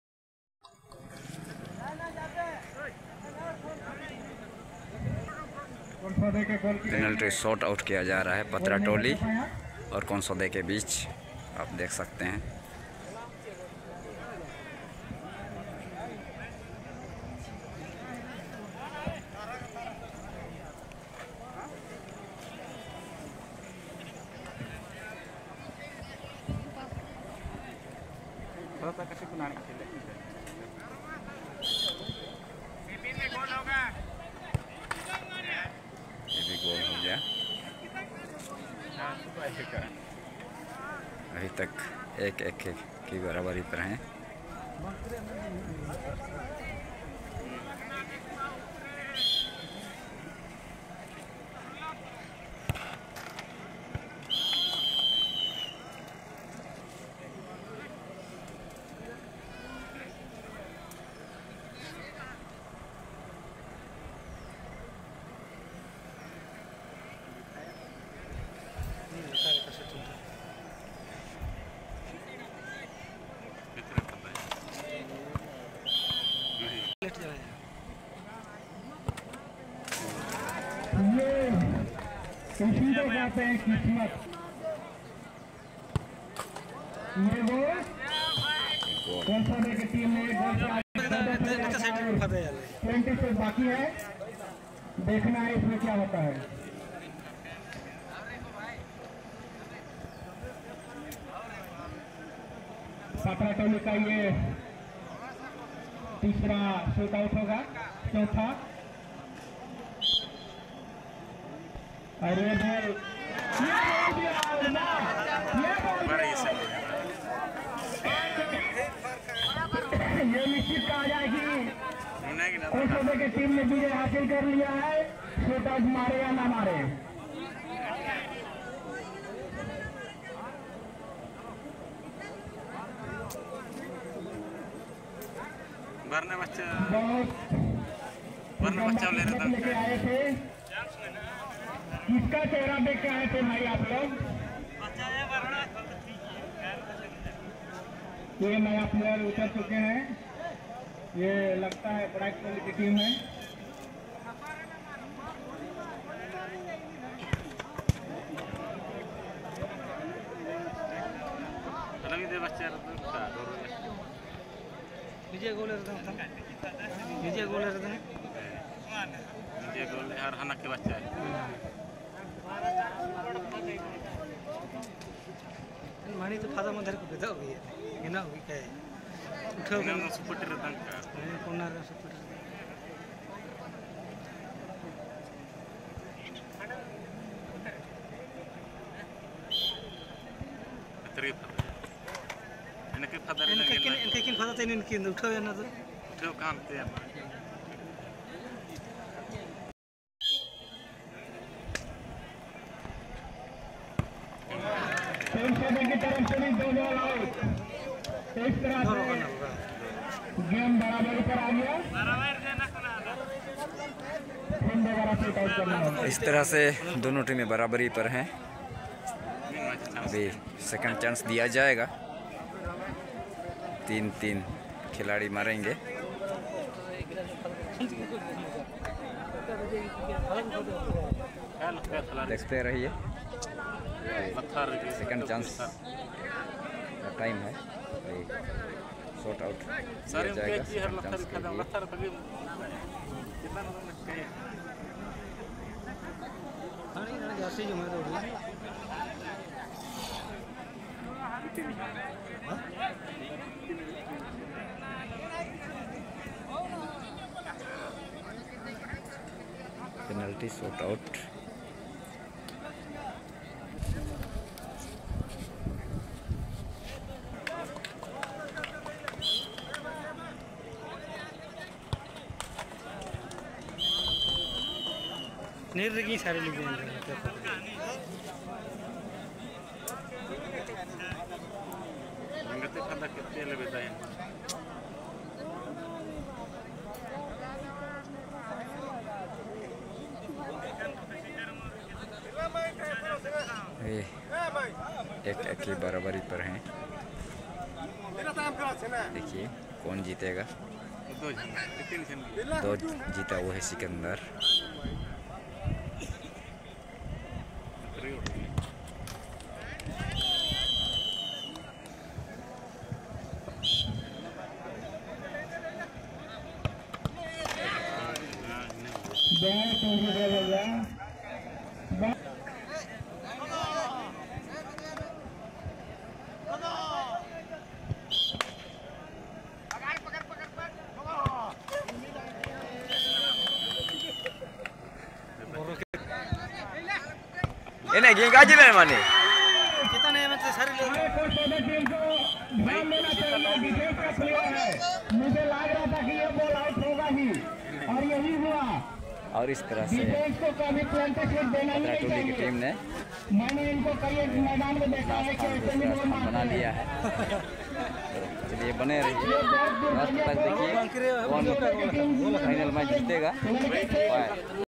penalty shootout||Konsode vs Patra toli||Football semi final Match||Soy,Bano||Jaimasih Marki Vlog
Bano Football semi final
footballtournament Bano,soy
Football Penalty shootout